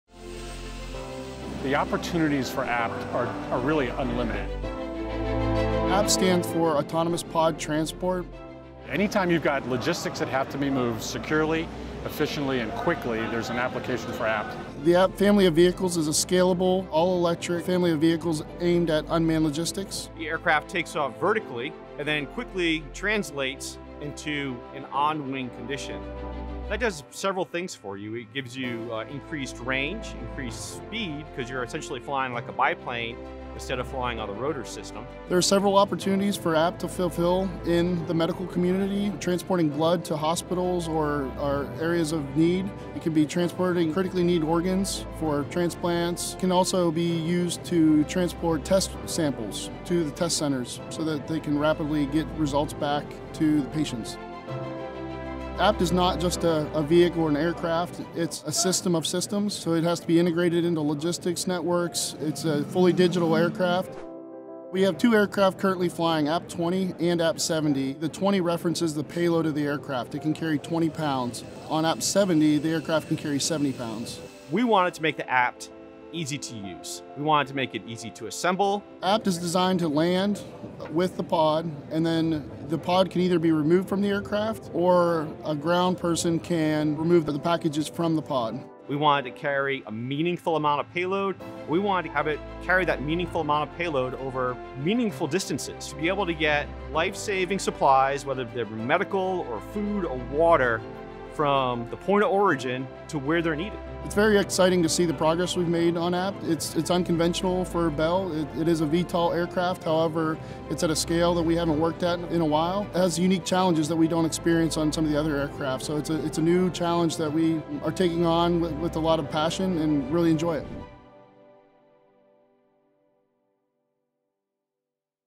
Bell APT Medical Mission
As the world embraces contact-less delivery more and more, Bell is working to provide the medical world a better solution. Meet the APT, an unmanned vehicle that moves like a helicopter and a plane, getting what you need there, faster.